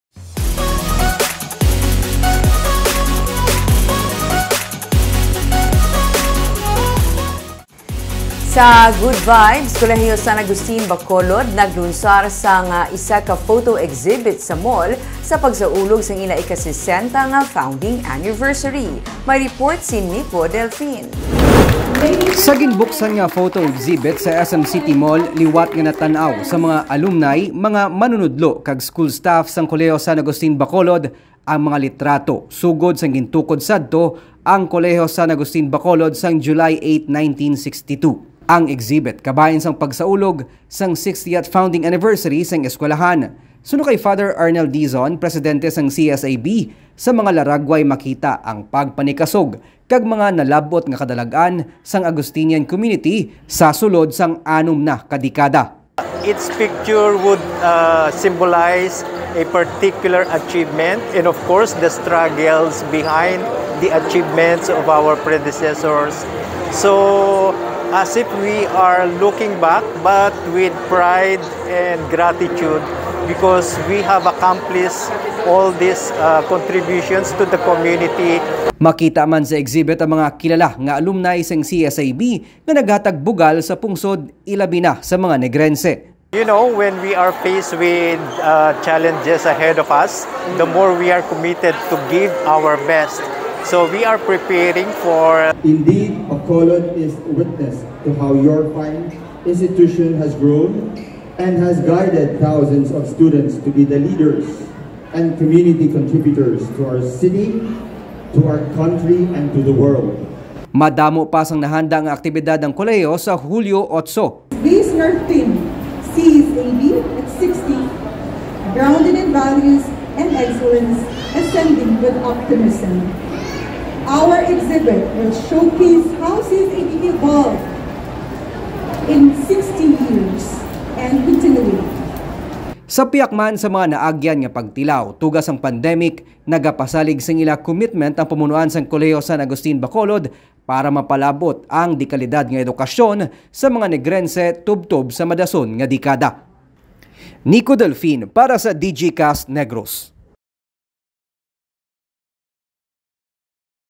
CSA-B 60th Founding Anniversary
Colegio San Agustin Bacolod nagasaulog sang ila 60th Founding Anniversary